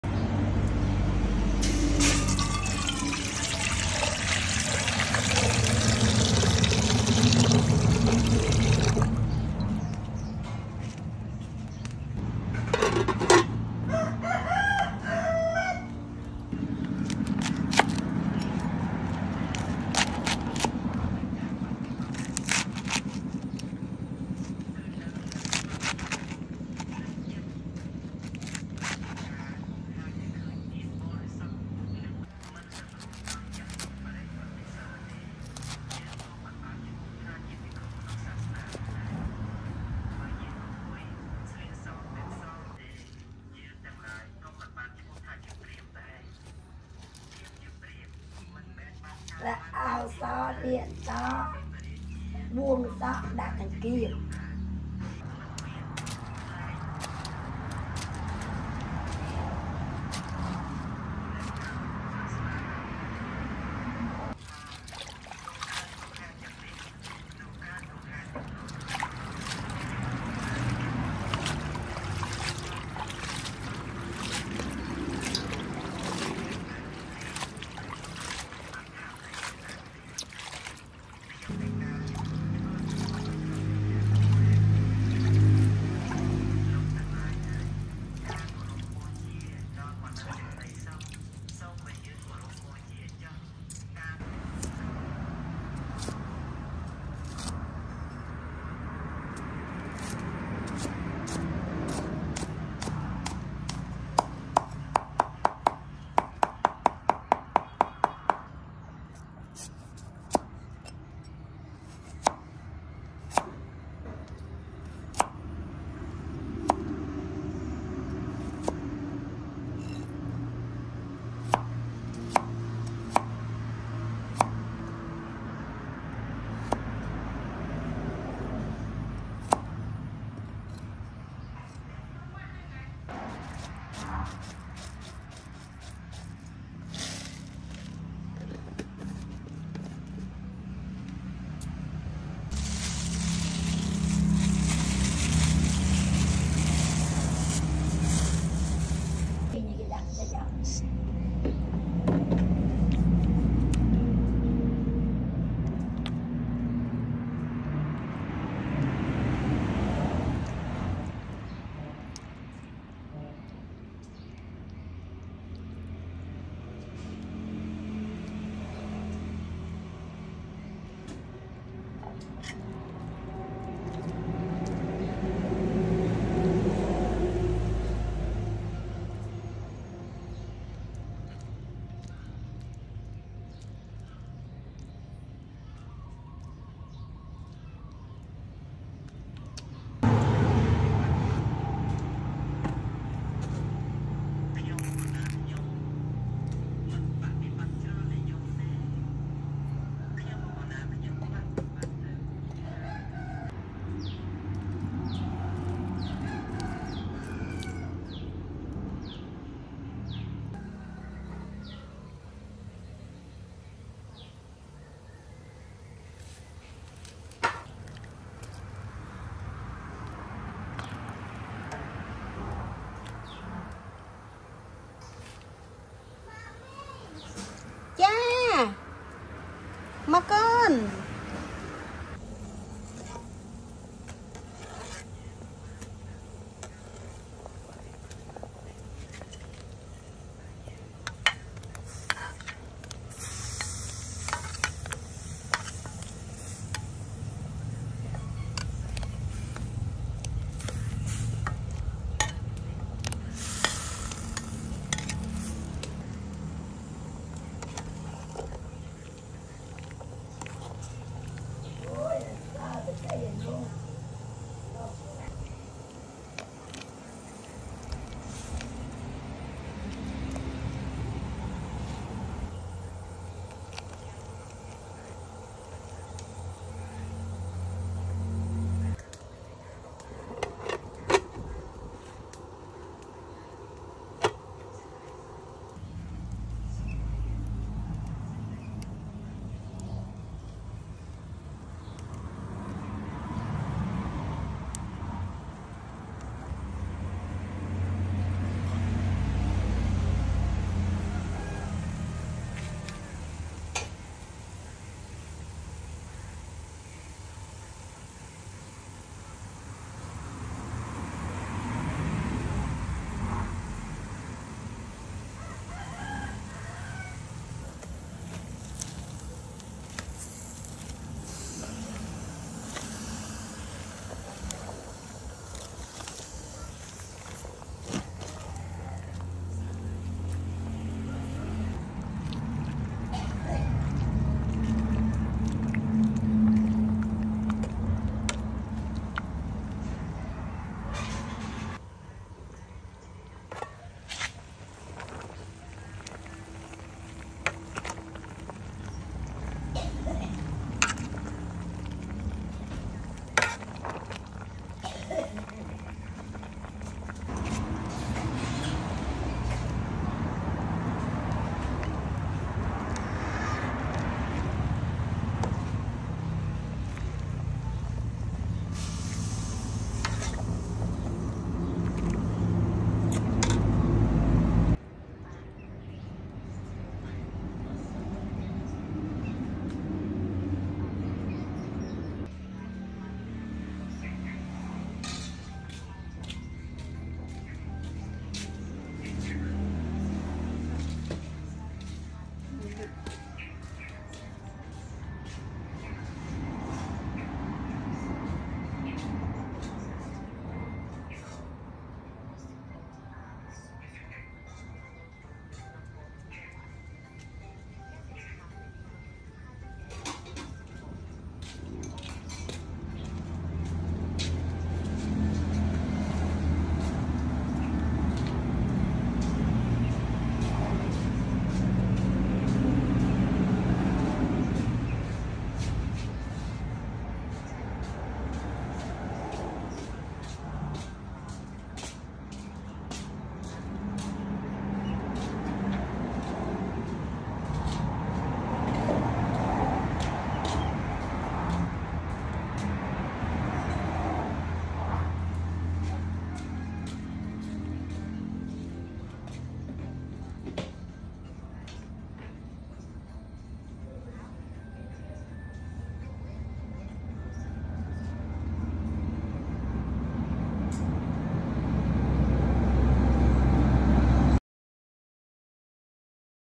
Spinach Soup With Fishes Ball
Hello everyone!How are you?Now I am back to see again with my new recipe.Today I cook Spinach Soup With Fishes Ball.This is very easy to cook and make you get healthy.Please watch my video to see the method.Thank you so much for your watching.Sending Love